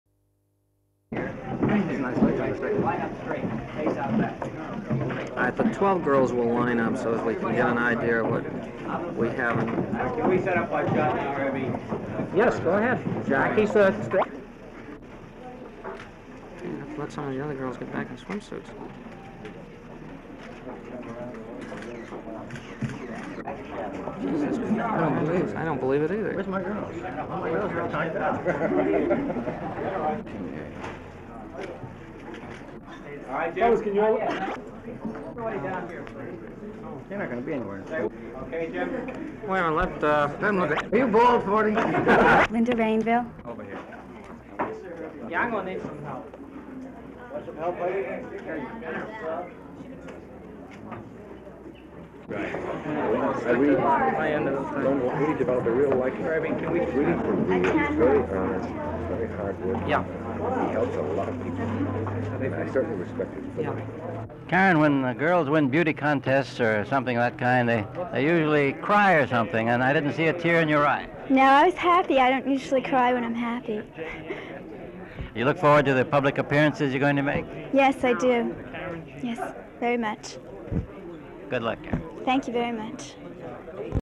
News Item (1e0e00a7-56d1-01af-3ad2-69b96c378acd)
(11 Apr 1973) 04/11/73   c0033604 - color
 new york:  miss photographer

 shows:  ny press photographers pick their "miss":  50 girls on stage various scenes:  winner #14 maureen mooney of hauppage li: runner up karen hochreiter:  receives flowers:  sof q&a:
 (shot 4/11 52ft)

 mooney, maureen
 hochreiter, karen
 beauty contests
 photography
 journalism - ny - ny
 grimsby, roger
 shalit, gene
 schwartz, hebb
 garelik, sanford
 dunn, tom

 52 ft / colo / rev
 opt trk
 350 ft / cl / rev / cuts